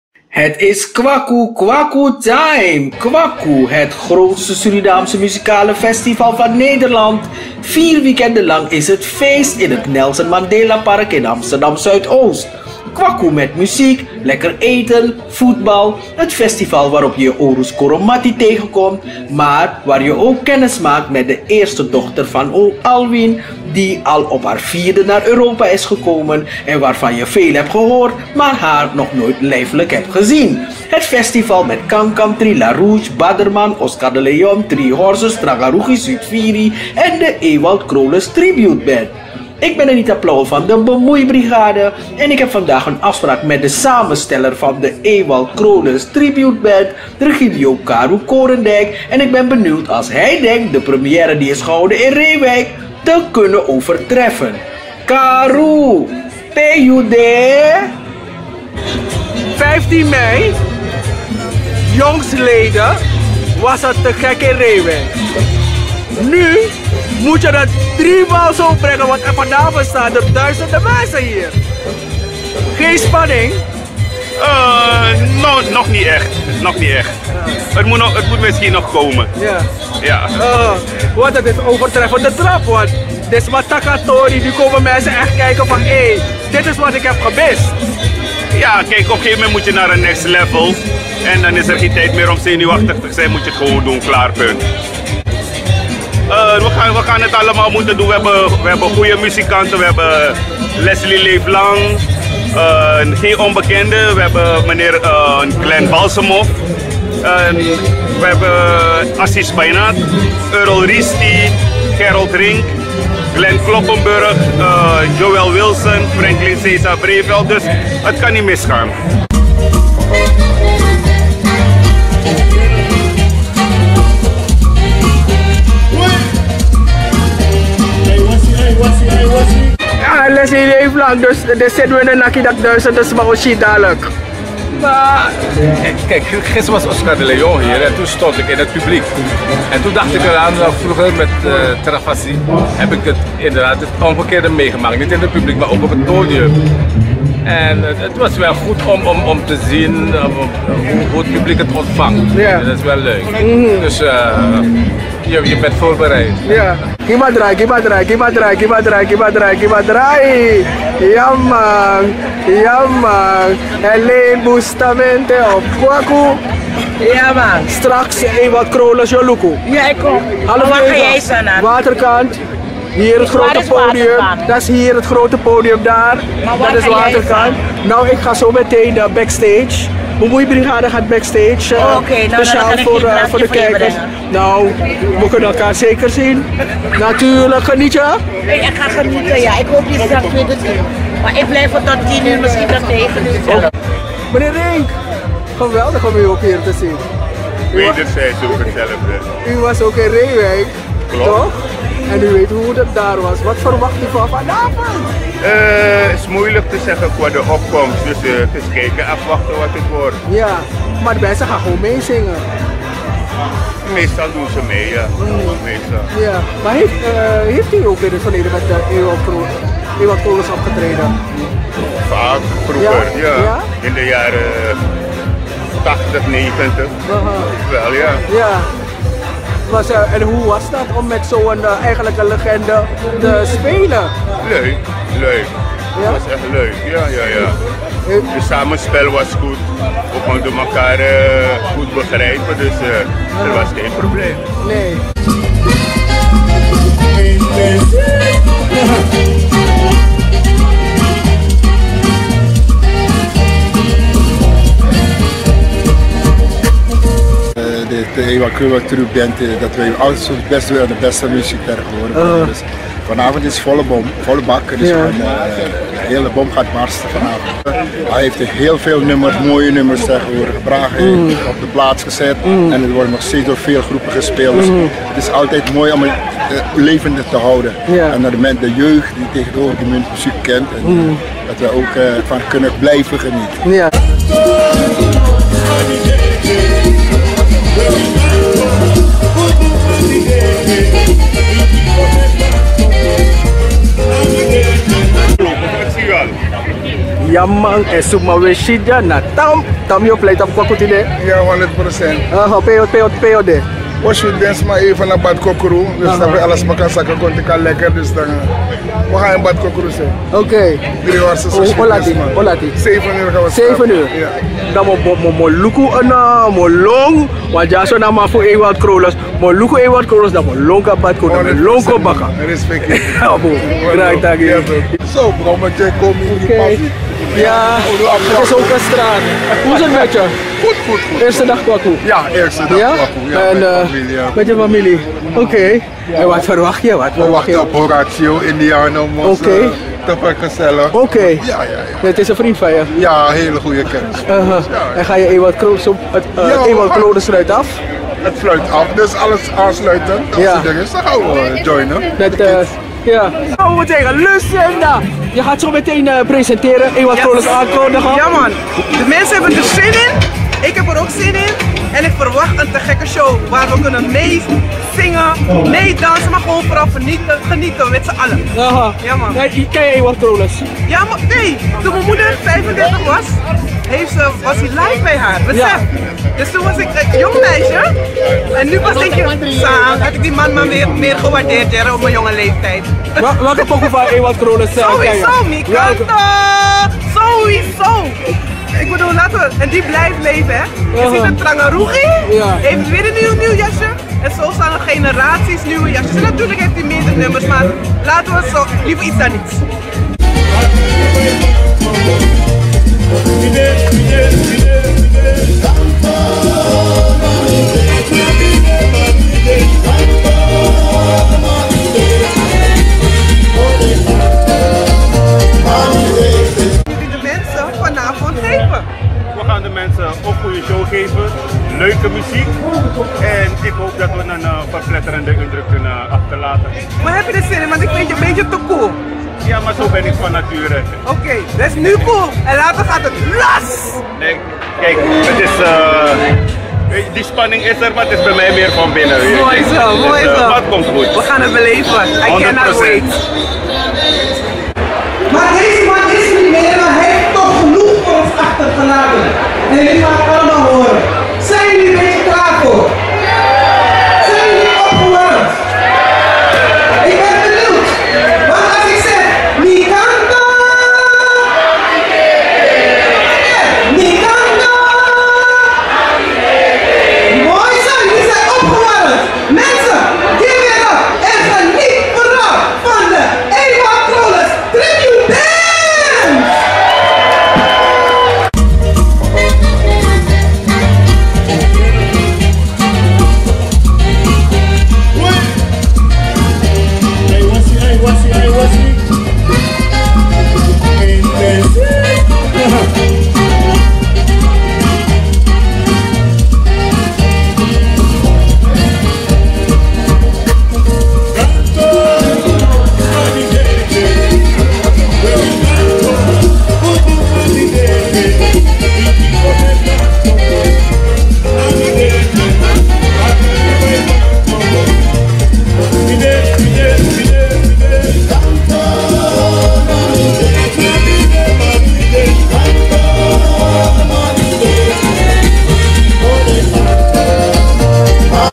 Avondje Kwakufestival met de Ewald Krolis Tribute Band en Lucinda Sedoc
Wie zijn de mensen die de Surinaamse muziek maken?
Wie zijn de creators van Surinaamse afkomst voor en achter de schermen.